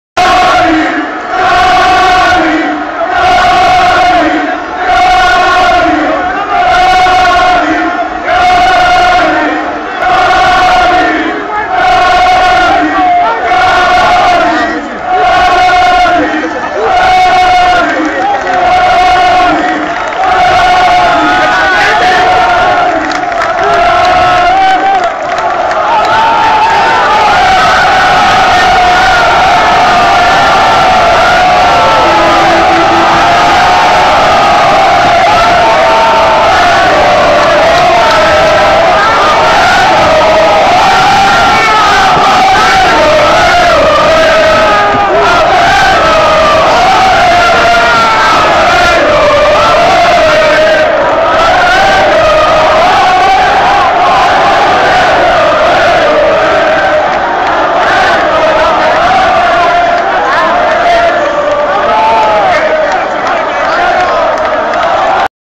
Llegada del autobús de Cádiz CF contra el R. Unión de Irún - 17 de mayo de 2009
Llegada del autobús de Cádiz CF al Estadio Carranza en el primer partido de la eliminatoria de Play Off de ascenso a Segunda División contra el R. Unión de Irún el 17 de mayo de 2009